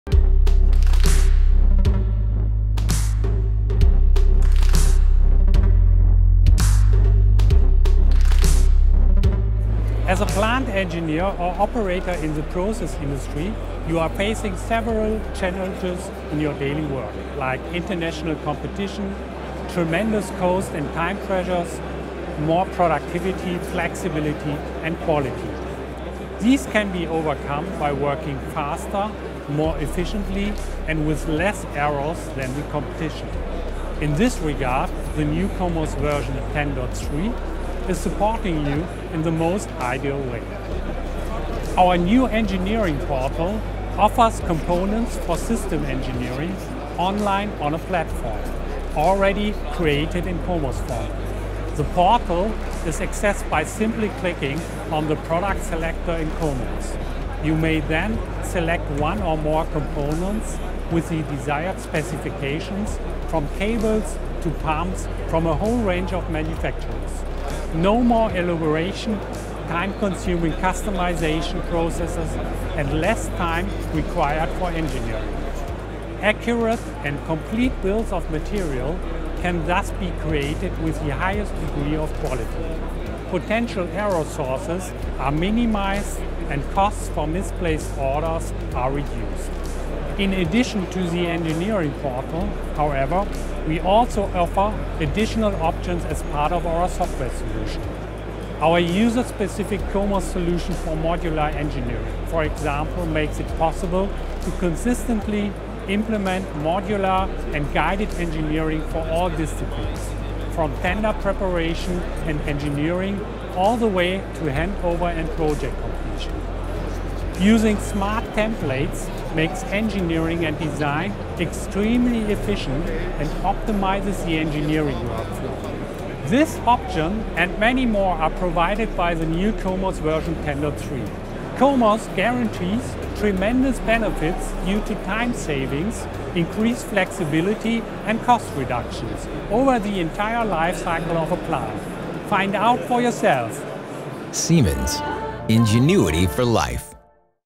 Highlights COMOS Version 10.3 - COMOS – Making data work. (English)
The new COMOS Version 10.3 is supporting you in the most ideal way. Our new Engineering Portal offers components for system engineering online on a platform – already created in COMOS format.